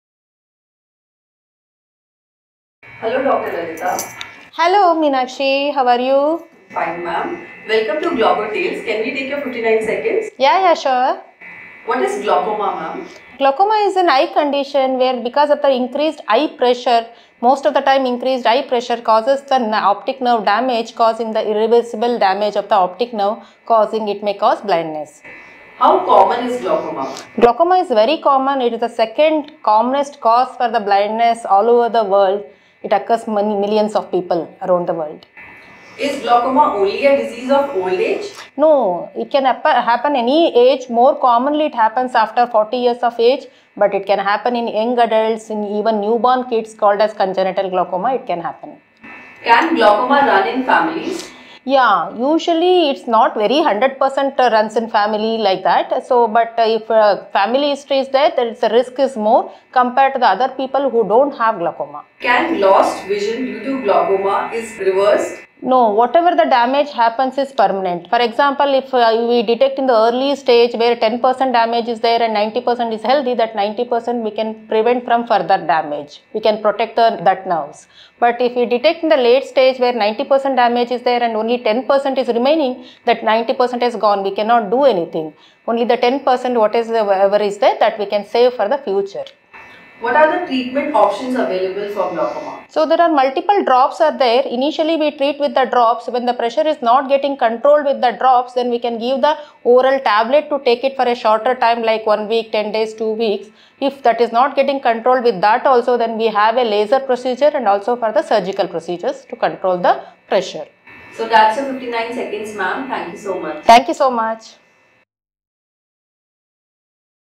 Glaucoma Awareness by Dr.Lalitha- Consultant (Glaucoma)- Sankara Eye Hospital , Bangalore
59 seconds with  Dr.Lalitha ( Consultant-Glaucoma), Sankara Eye Hospital - Bangalore

glaucoTales glaucoma awareness